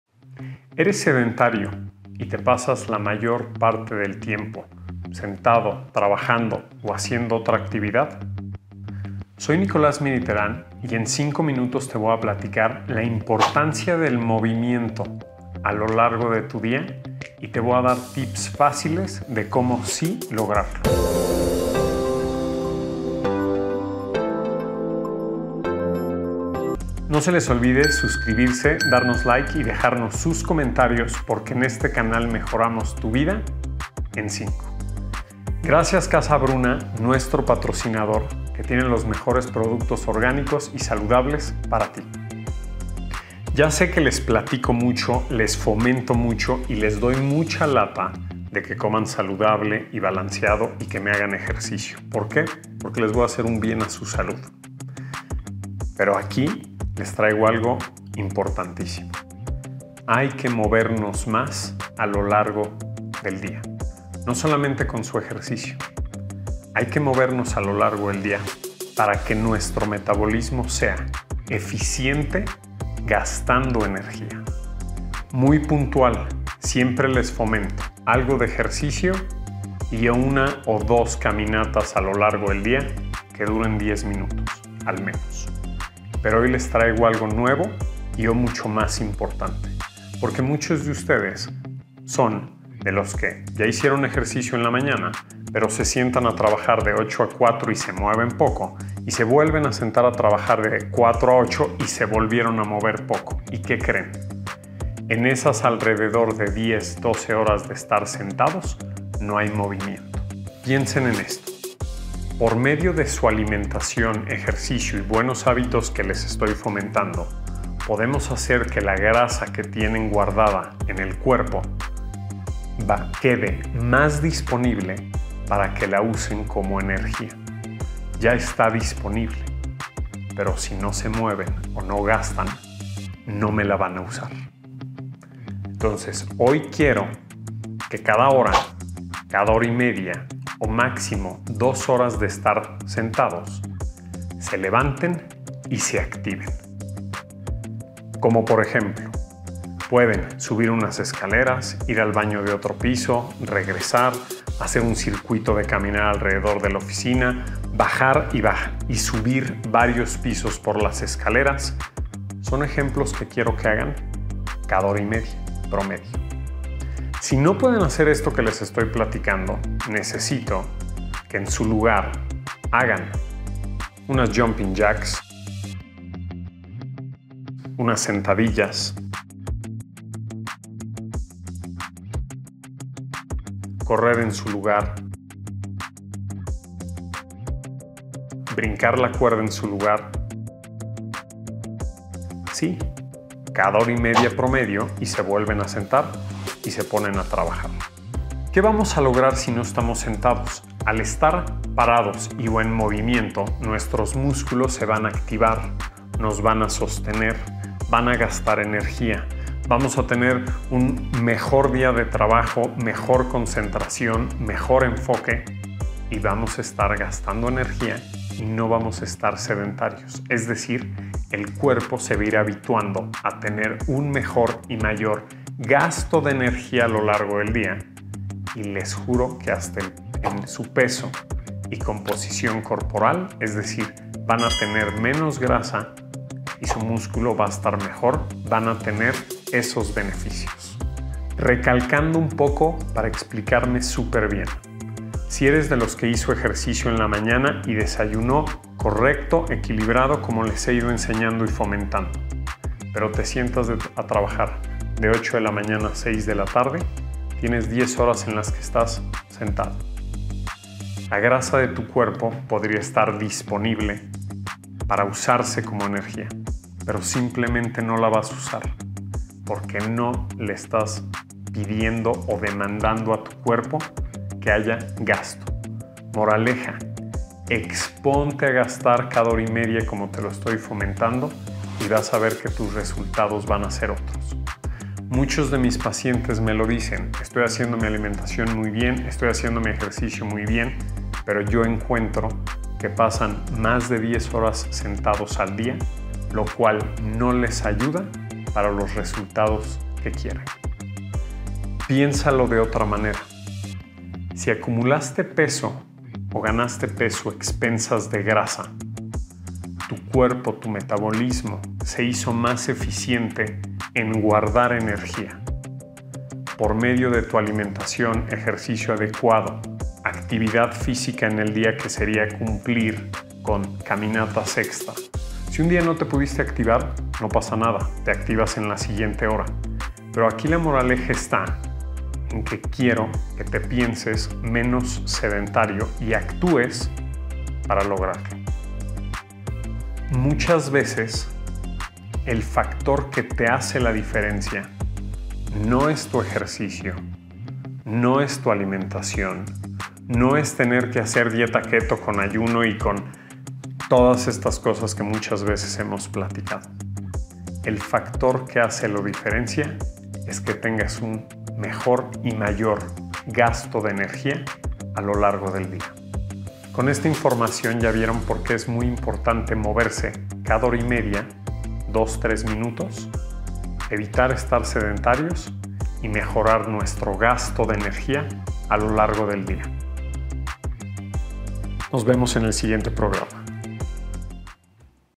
Prueba este Hábito antes de Hacer Dietas o Gimnasio | Nutrición En Cinco | En5.mx
Mantenerte en movimiento a lo largo del día te cambiará la vida.
Esta es la importancia de activarte cada hora del día para que puedas bajar de peso y tener un cuerpo sano.

0:00 - Intro
0:43 - La importancia de moverse
1:15 - Qué tanto movernos al día
1:26 - Un mejor hábito a lo largo del día
2:46 - Opciones si no puedes caminar
3:18 - Beneficios de estar en movimiento
4:01 - La dieta y el gimnasio no son suficientes
5:01 - ¿Por qué es importante moverse?
5:39 - El factor #1 para bajar de peso

¡Compártelo con tu persona favorita!

REDES EN CINCO

Nuestro experto:

NICOLÁS MIER Y TERÁN
Nutriólogo